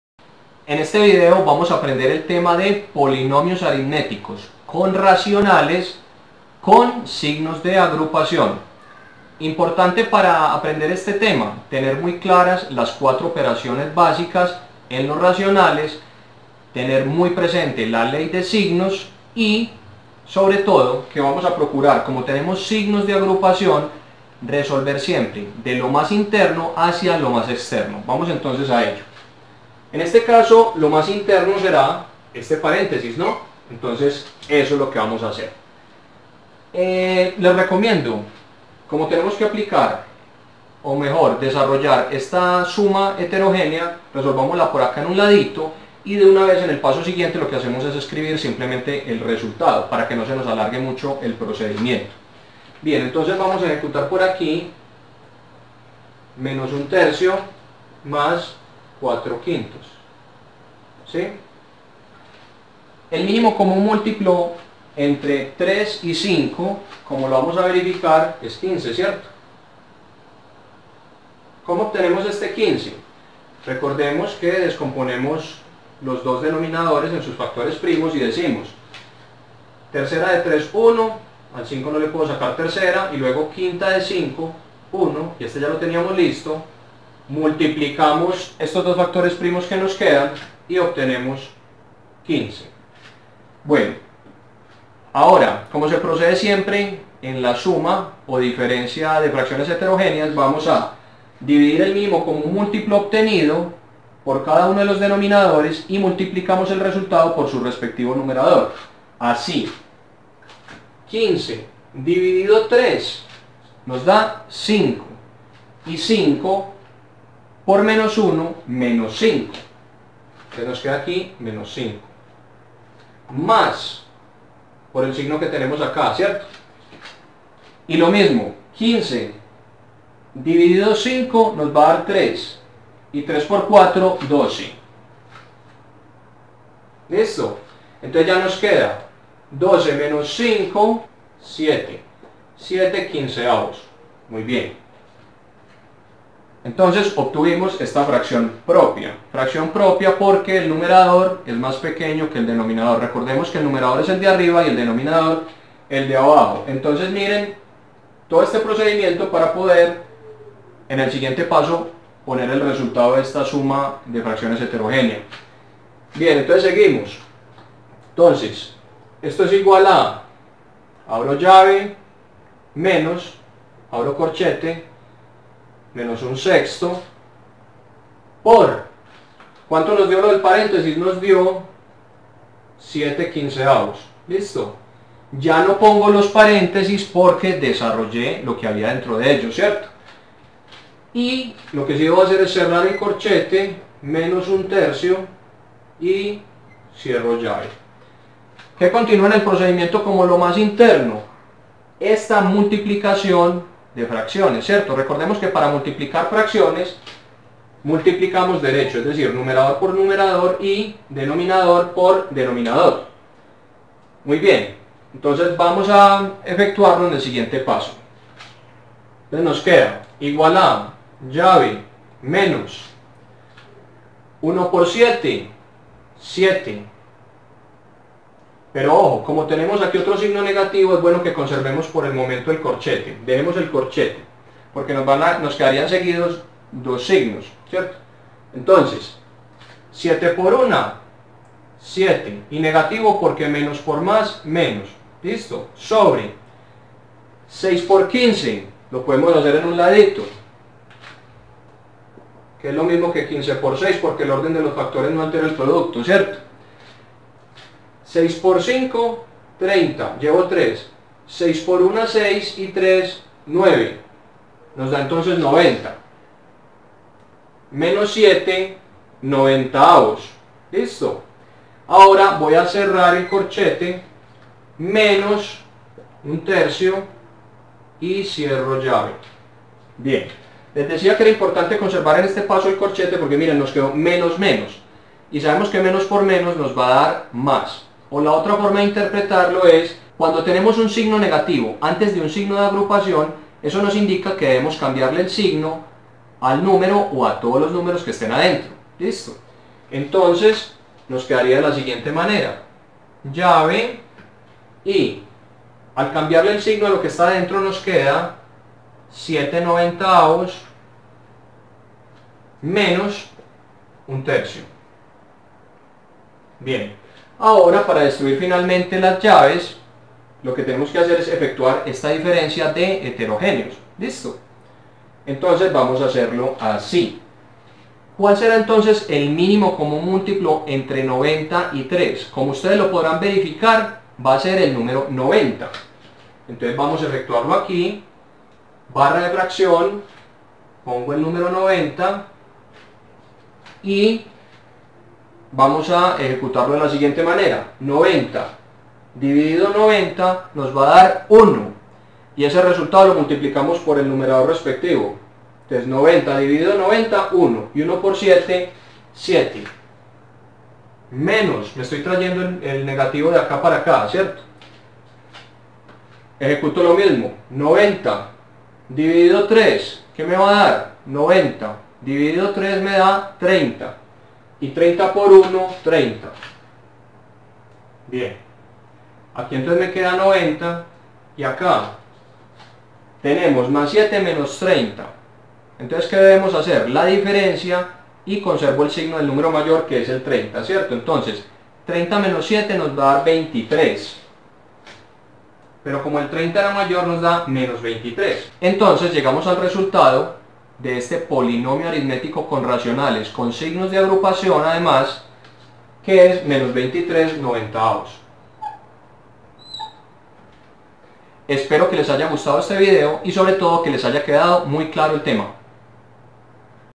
POLINOMIOS ARITMETICOS CON RACIONALES con signos de agrupacion [mas facil imposible !!!]
Aprende fácil y rápido el tema de POLINOMIOS ARITMETICOS CON RACIONALES con signos de agrupacion.